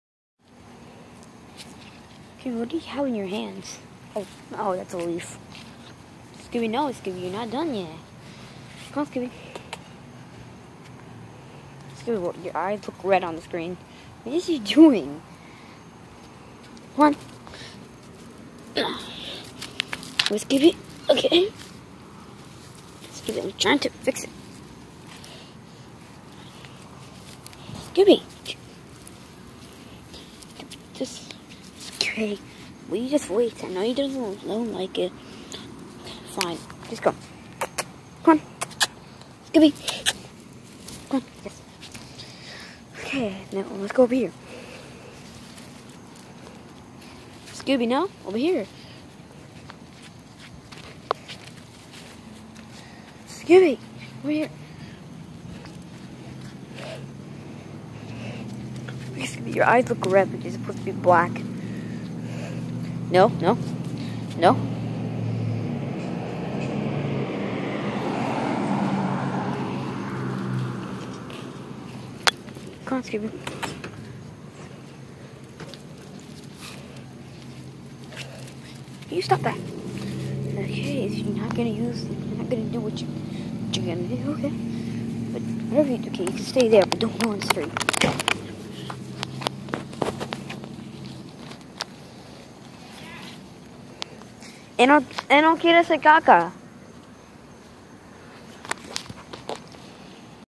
Walking Scooby at night #1
Nothing much just walking my dog.